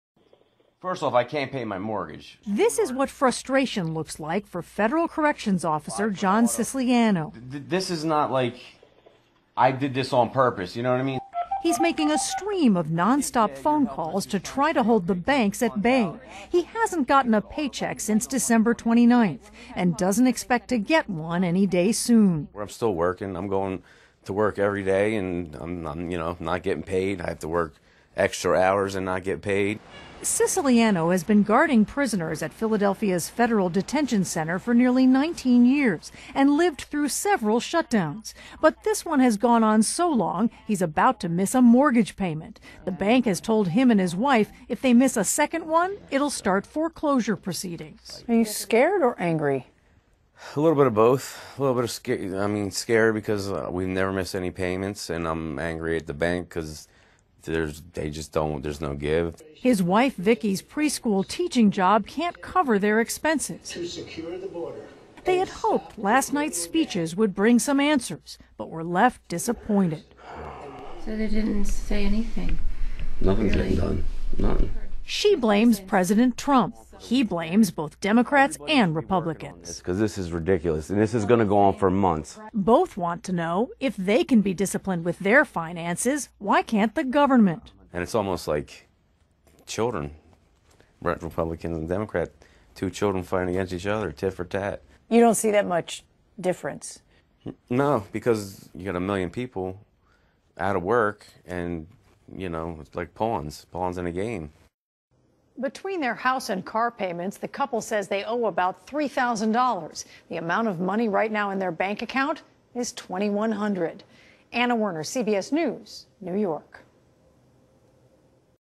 Corrections Officer At Risk Of Foreclosure As Shutdown Drags On
ᐸpᐳAs the&nbsp;partial government shutdown&nbsp;drags on 800000 federal workers are forced to live off savings and what's left from their last paychecks including New Jersey resident and federal corrections officer John Siciliano.&nbsp;ᐸ/pᐳ